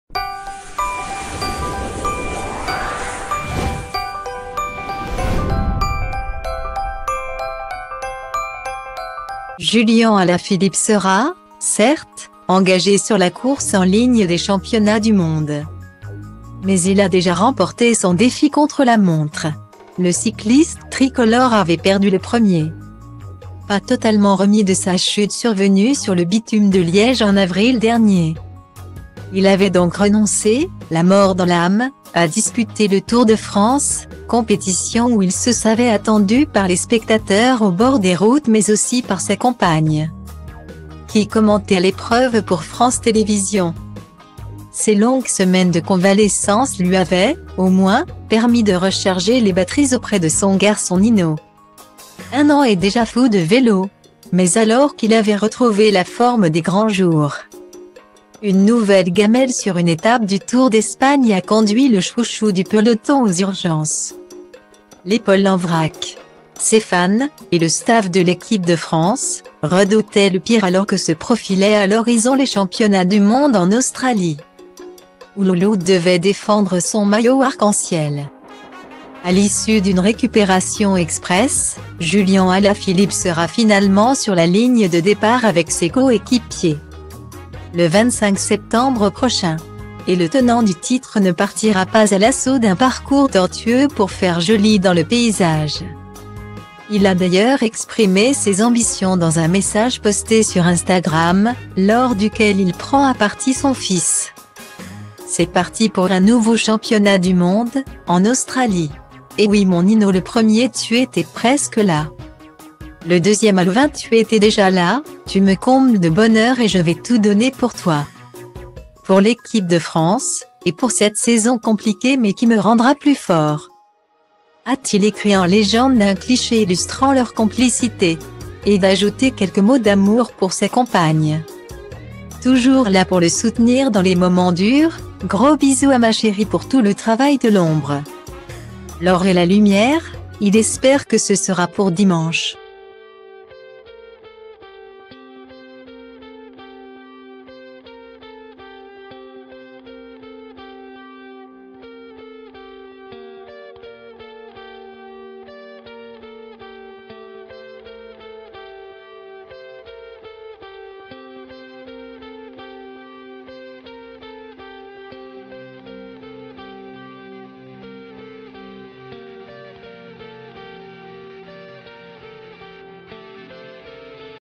« Merci pour le travail de l'ombre » : la poignante déclaration de Julian Alaphilippe à sa compagne
« Merci pour le travail de l'ombre » : la poignante déclaration  de Julian Alaphilippe à sa compagne Marion Rousse
Julian Alaphilippe sera, certes, engagé sur la course en ligne des championnats du monde,mais il a déjà remporté son défi contre-la-montre.